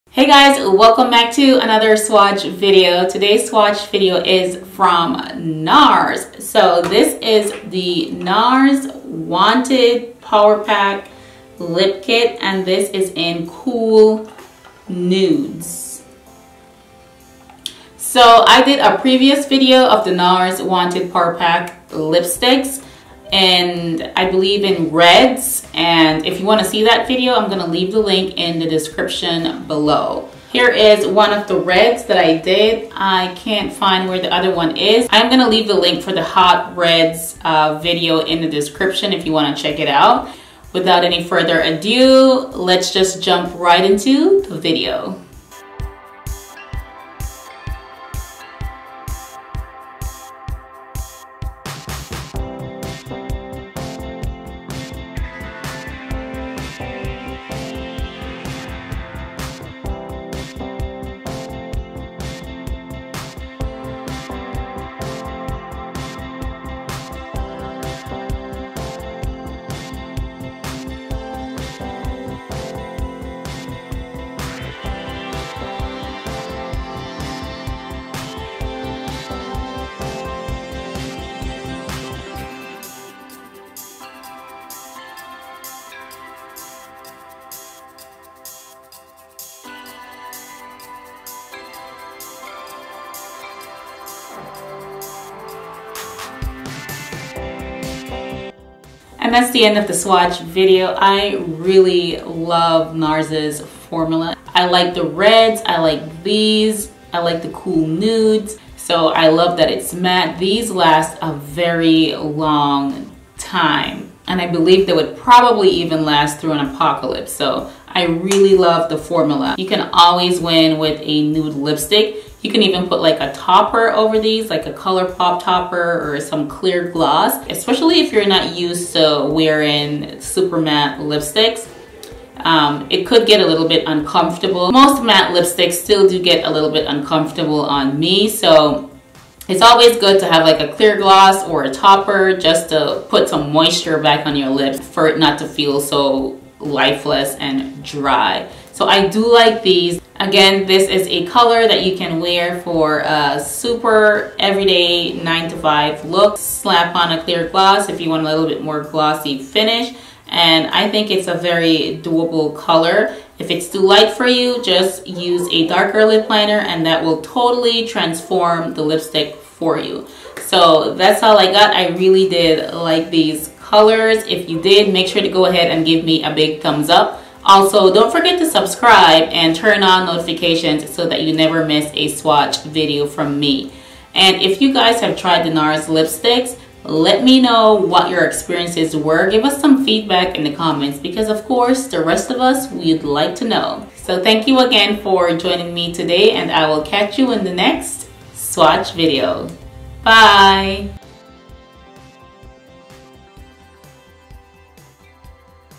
NARS Wanted Power Pack Mini Lip Kit NUDES Lipstick Swatches | PuckerUpBabe
So here is round two of the NARS Wanted Power Pack Mini Lip Kit in NUDES.

Shades featured:
NARS Le Freak
NARS American Woman

Let me know what you guys think in the comments. What other lipstick swatches would you like to see?